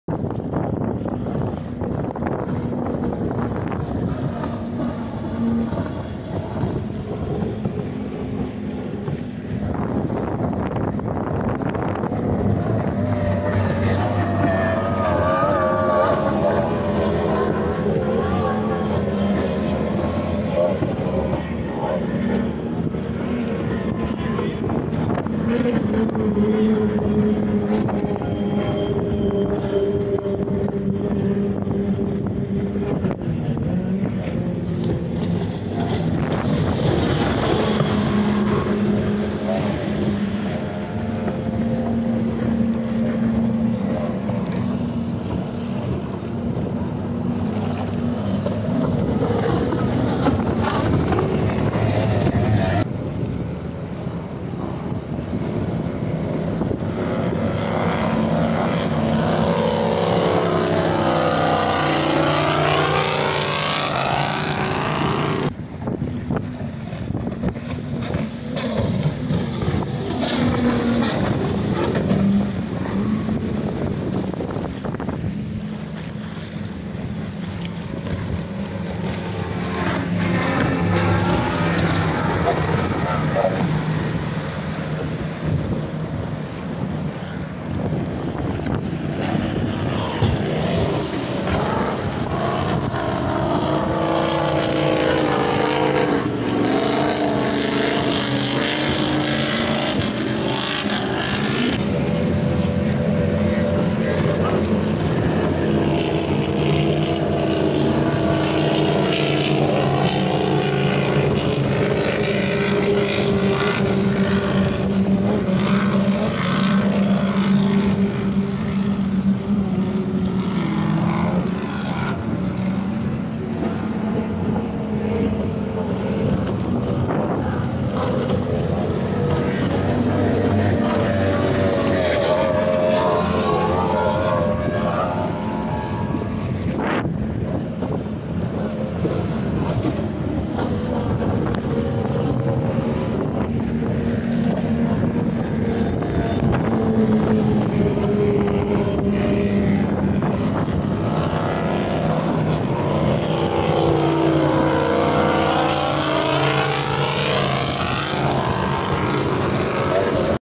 Offshore Championship Race
may 2021 offshore championship race @ Lummen ski in Belgium
LIFEONWATER was the clear winner today